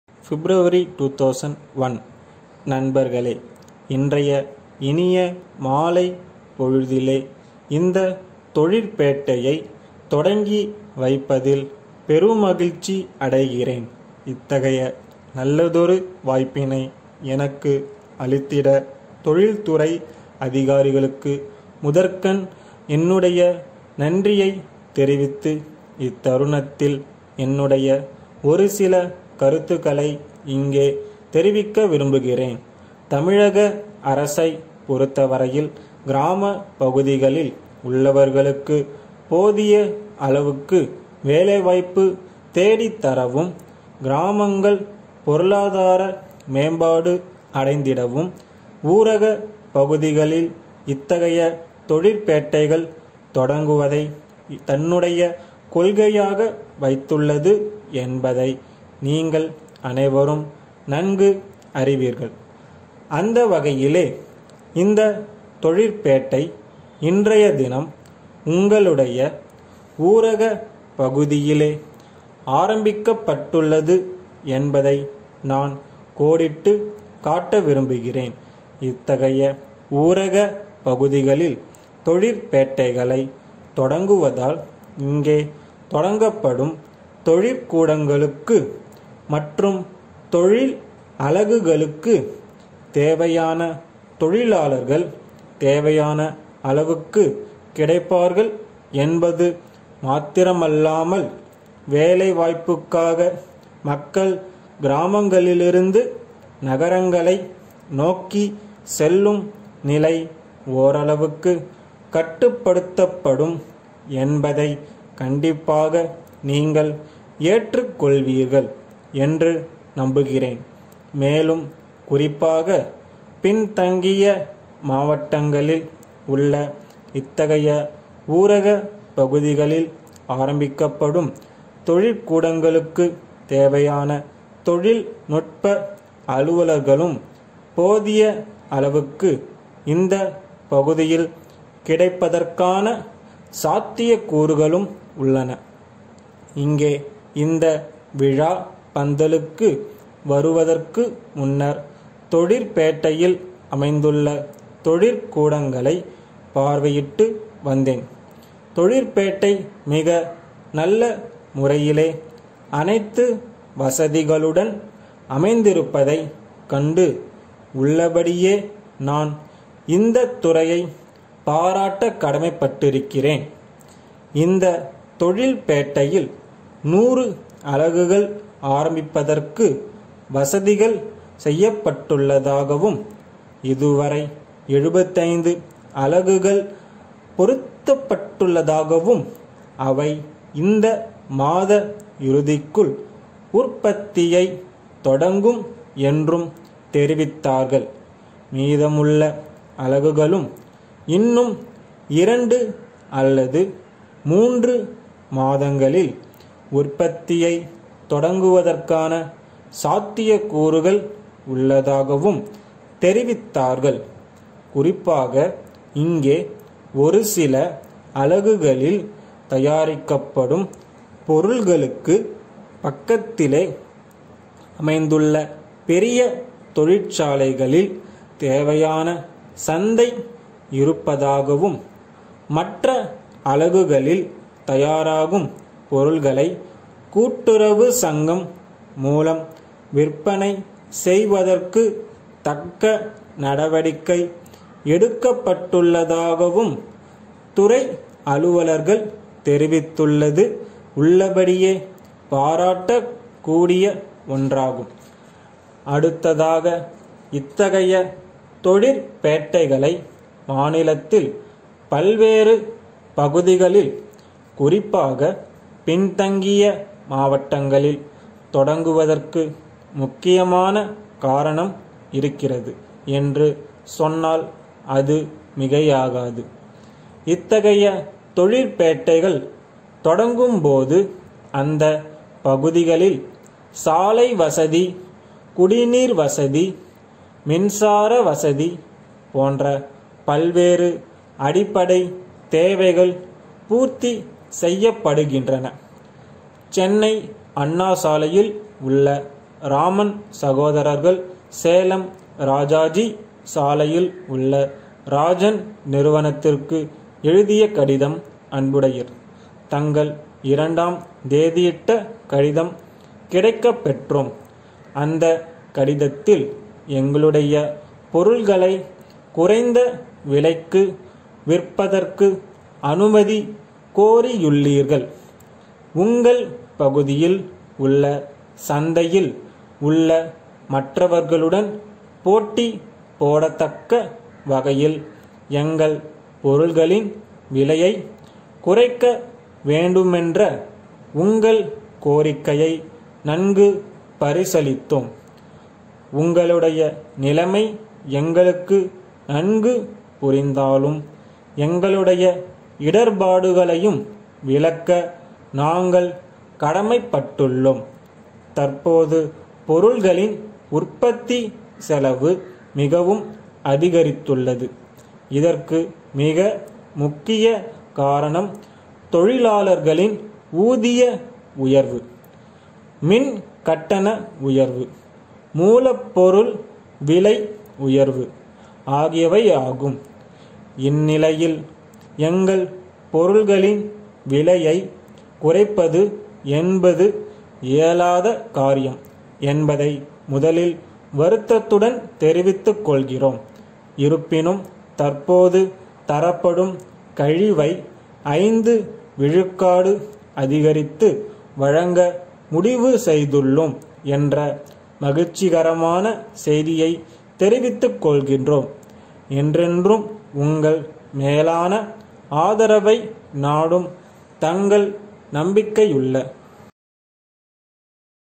Tamil Senior Shorthand Speed / 90wpm / Feb 2001
Tamil Shorthand speed practice for senior students.
Speed time within 7 minutes.
All Tamil and English speeds will be upload frequently.
Any doubts or regards contact to below groups.

Send Feedbacks in the comments